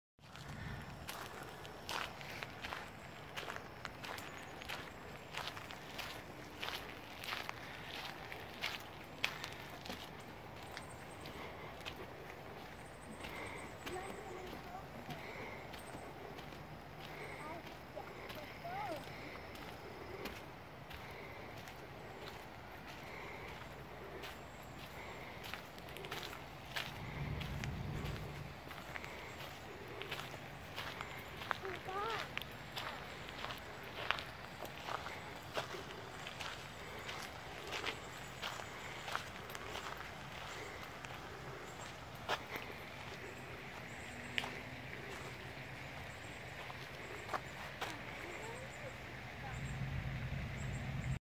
Crane walking across a bridge in the park.
Walking right behind this crane on the bridge while filming..